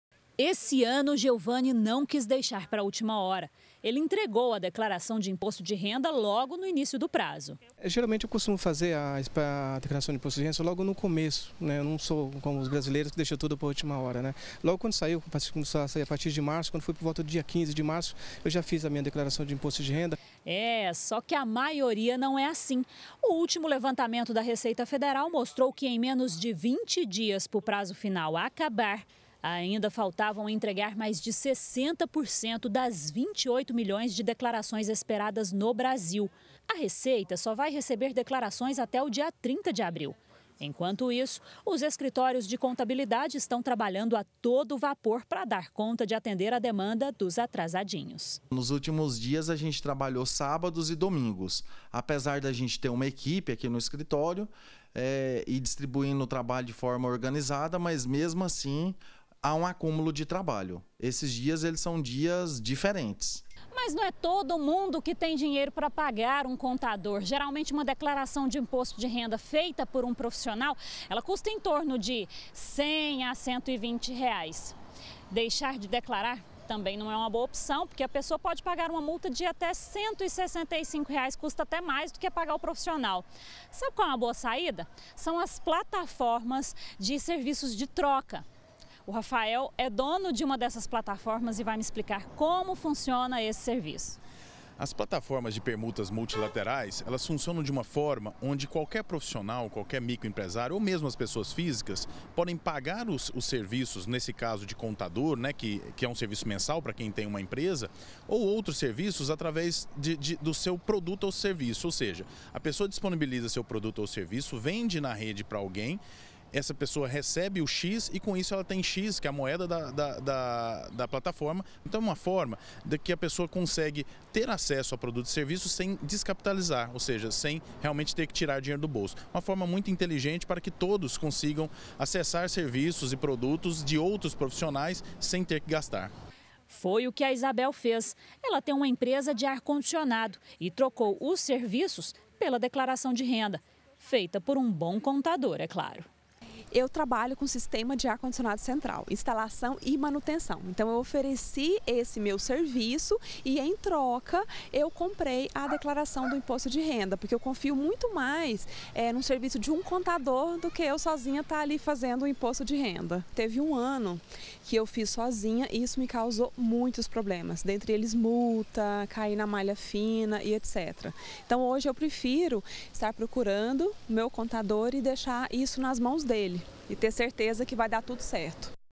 GA - Ultima semana para declarar IR - 23-04-2018
O prazo para a entrega do imposto de renda acaba no próximo dia 30. E os escritórios de contabilidade estão abarrotados de tanto serviço.